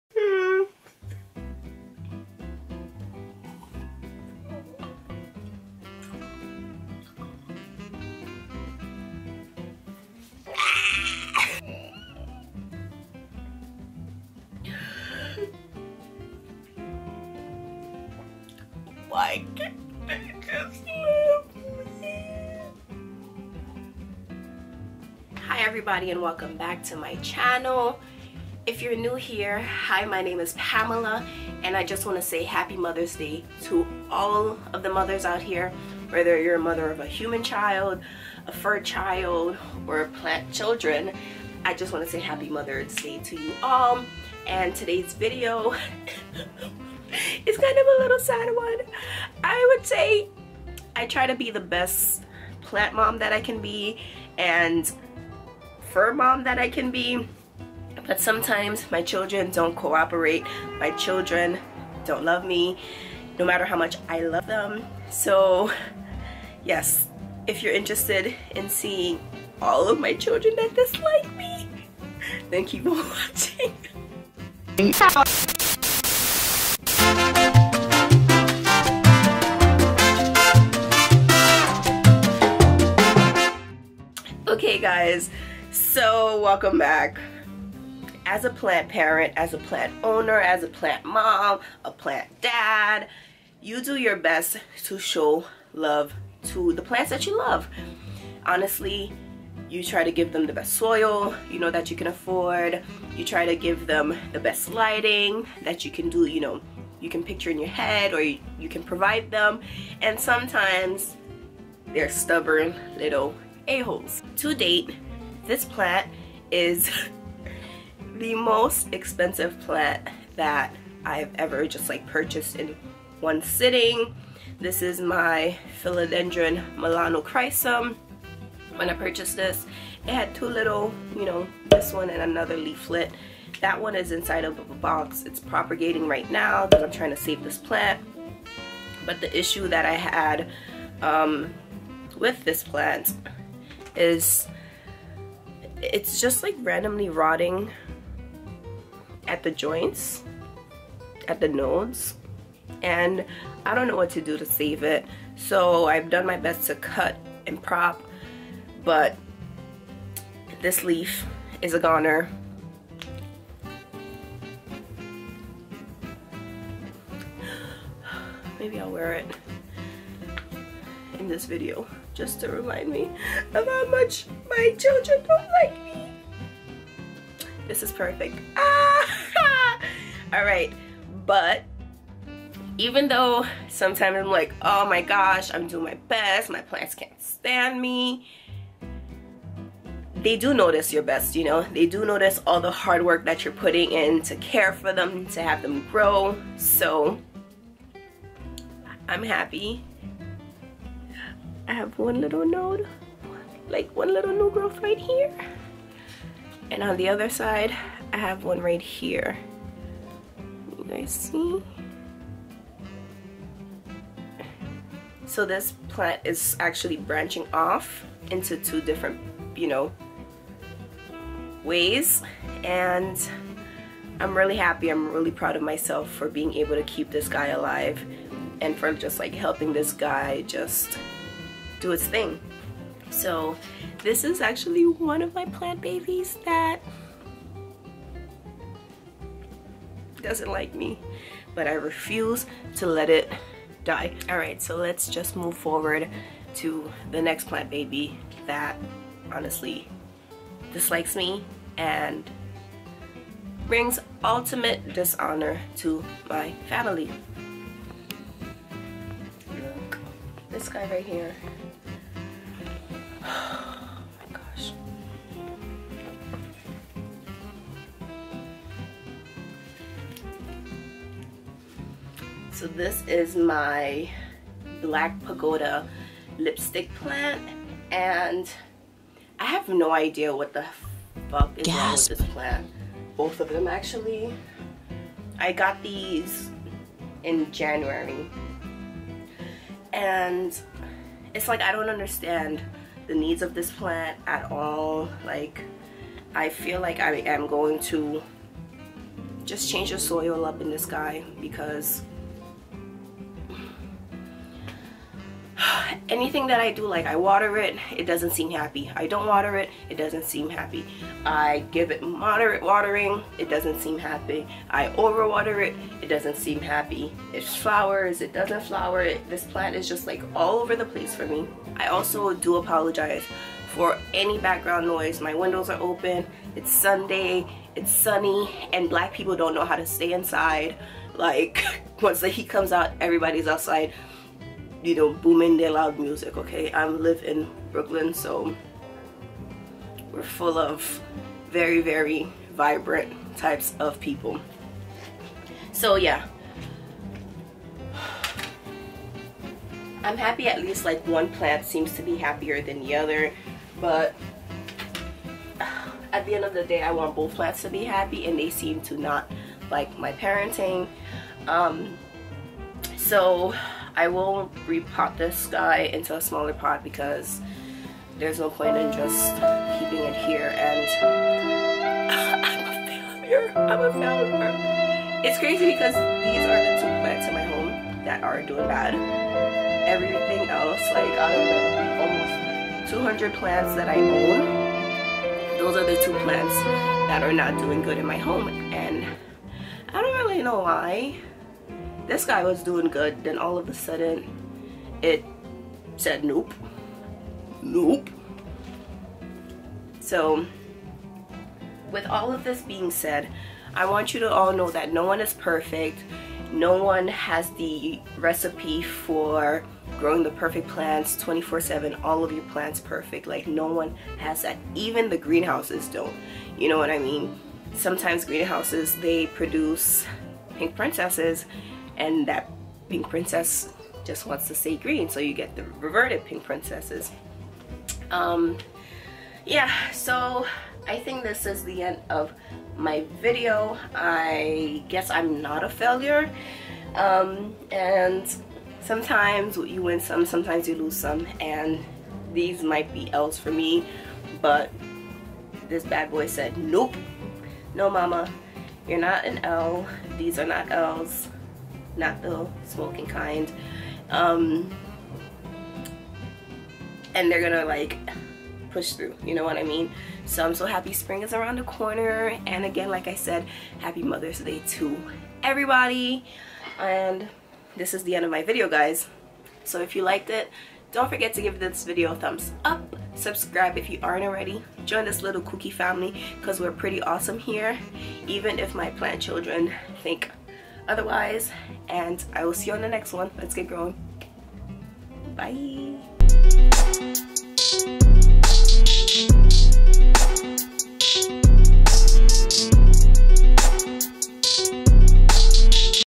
some of my children HATE me | Happy Mothers Day
some of my children hate me. no matter how much love care and attention i give. they're never satisfied. happy mothers day to all of the plant human & fur parents out there. i hope you enjoyed your sunday with your loved ones.

Personal IG: PamellaPierre
Plant IG: HauzPlant

Products Mentioned:

My Filming Equipment:
Mic: Rode VideoMicro
(cheaper Mic alternative)